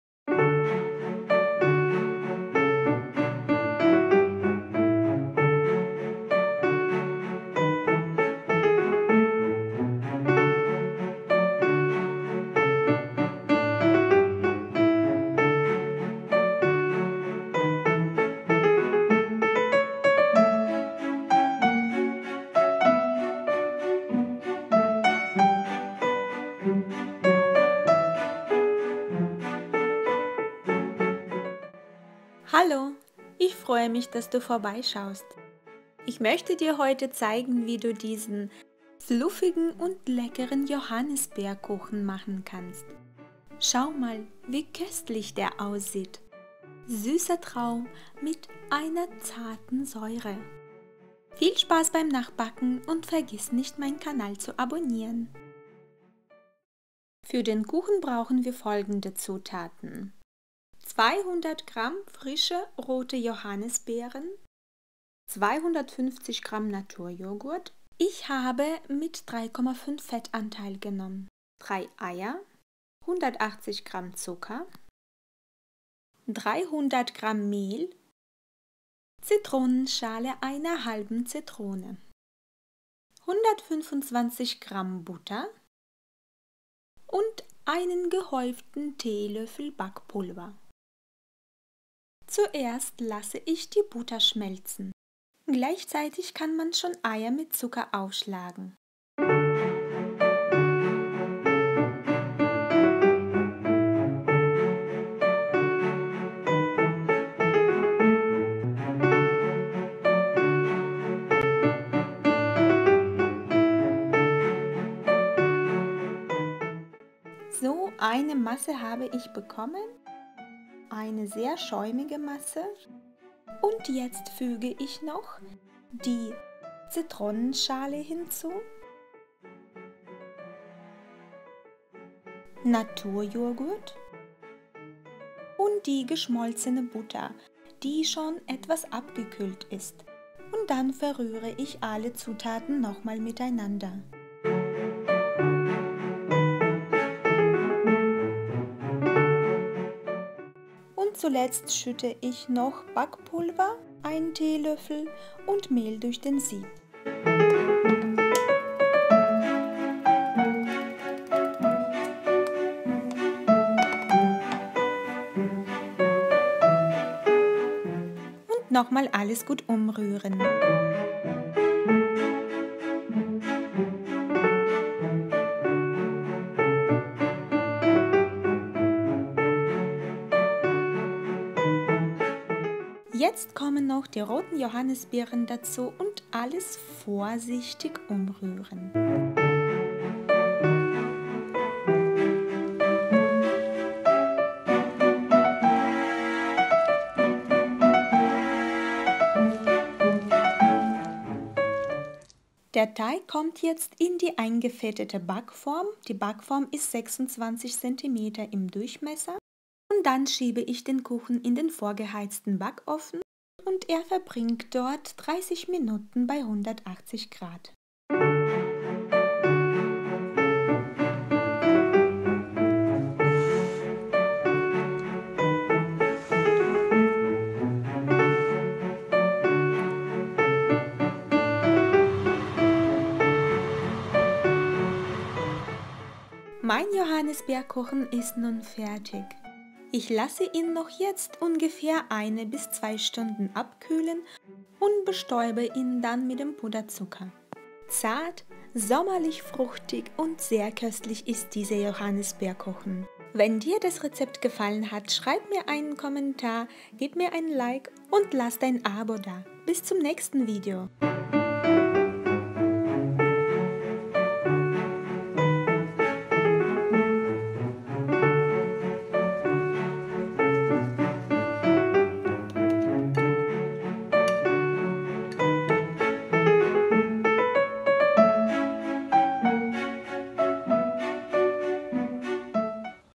Zarter, leckerer, saftiger JOHANNISBEERKUCHEN | Das beste Rezept
Köstlicher Johannisbeerkuchen - süßer Traum mit zarter Säure. In diesem Video findest du ein Rezept von einem sehr leckeren Johannisbeer-Jogurt-Kuchen. Statt Johannisbeeren kann man auch andere Beeren nehmen. Dieser Kuchen mit roten Johannisbeeren ist sehr soft, gelingt immer und sehr schnell zubereitet. Ein perfekter Kuchen zum Kaffee.

Zutaten

200 g rote Johannisbeeren
250 g Naturjoghurt
3 Eier
180 g Zucker
300 g Mehl
125 g Butter
Zitronenschale einer halben Zitrone
1 Teelöffel Backpulver